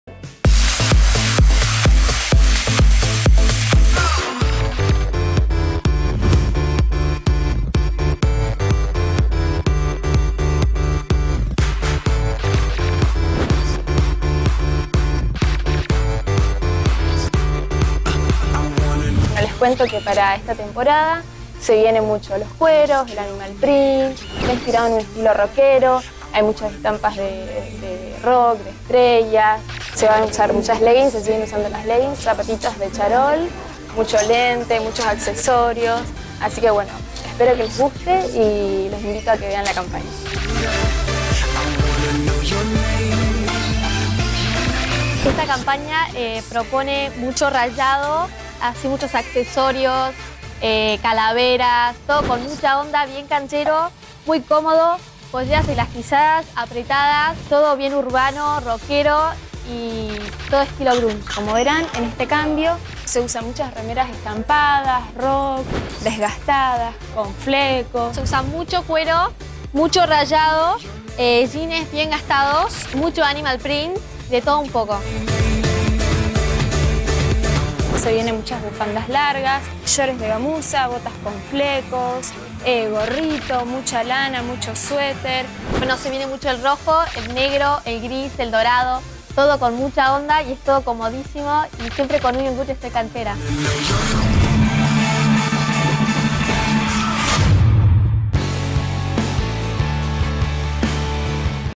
Campaña Otoño Invierno | TENDENCIA 2012
Las modelos Anara Moreira y Milagros Gregorini nos cuenta lo que se viene para esta temporada Otoño Invierno 2012. Seguinos en TWITTER FACEBOOK TODOS LOS DOMINGOS A LAS 19 HS POR CANAL 9 [ARG].